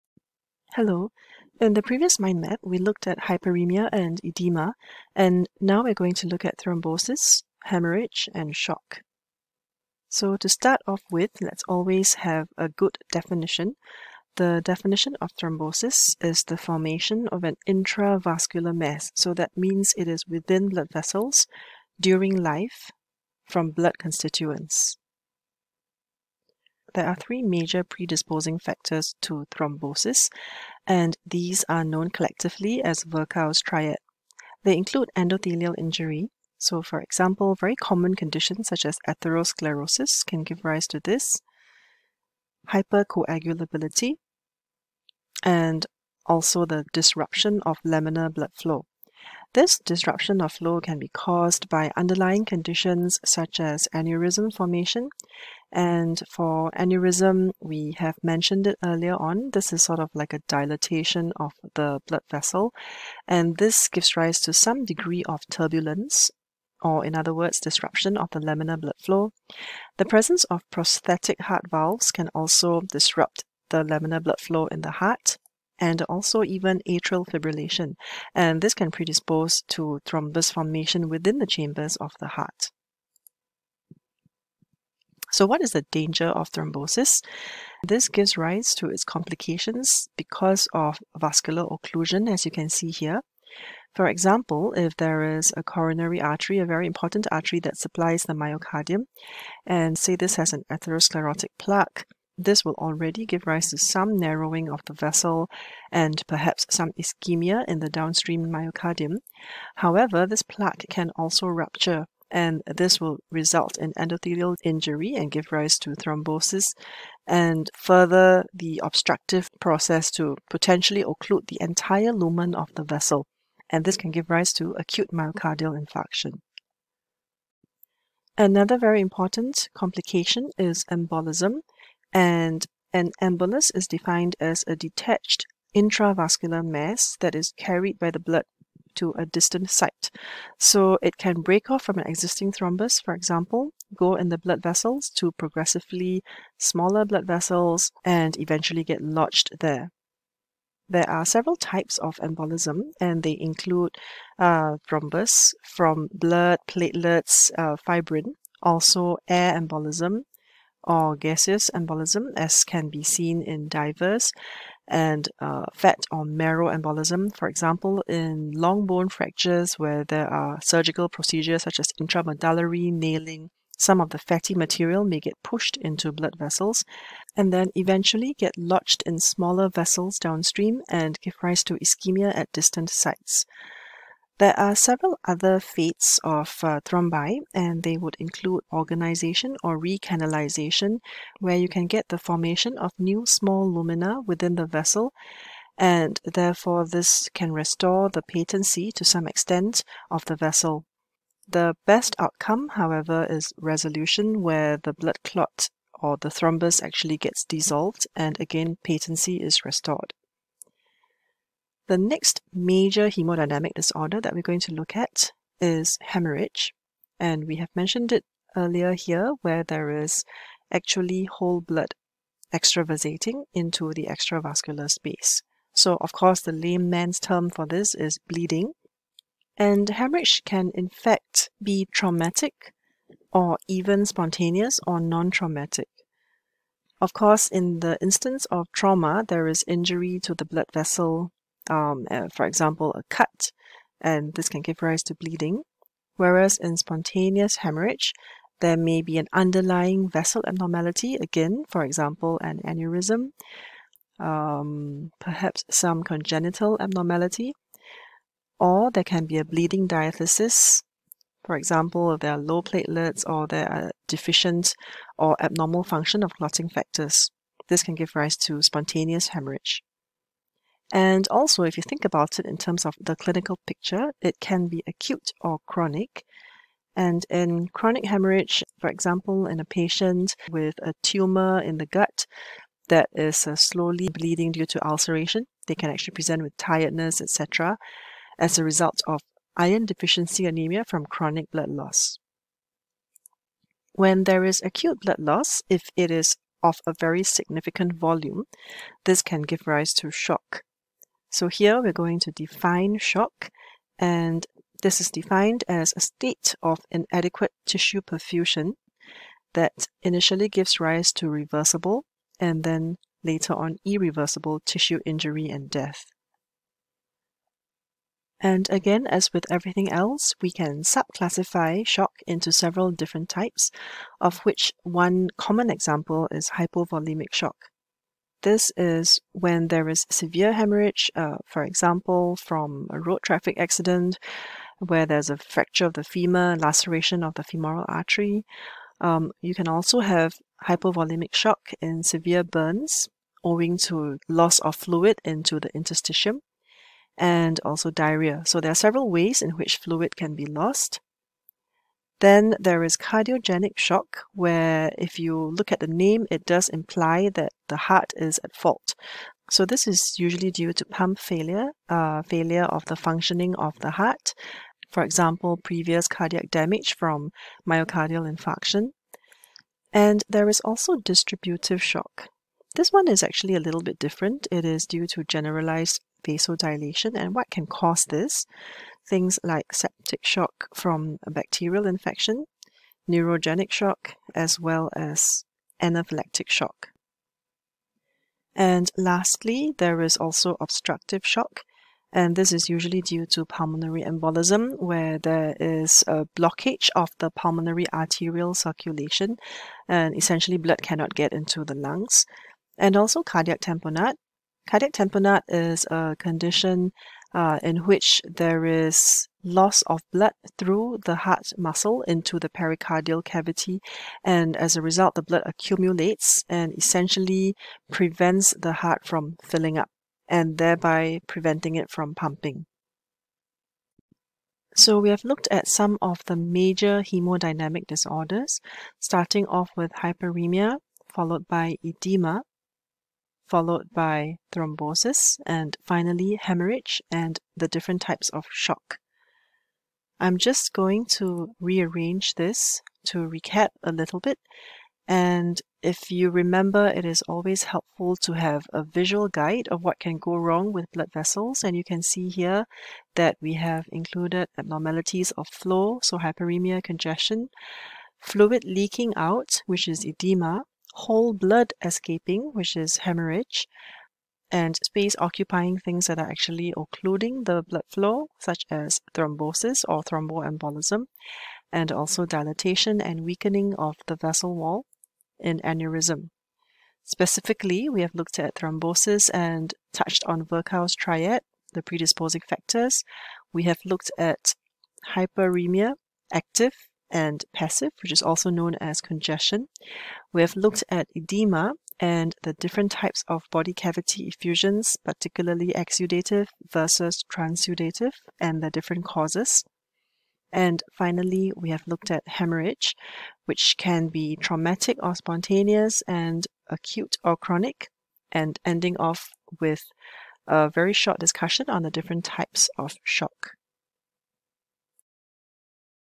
Haemodynamic Disorders II: Thrombosis, Haemorrhage and Shock
Mindmap overview of some major haemodynamic disorders, part 2.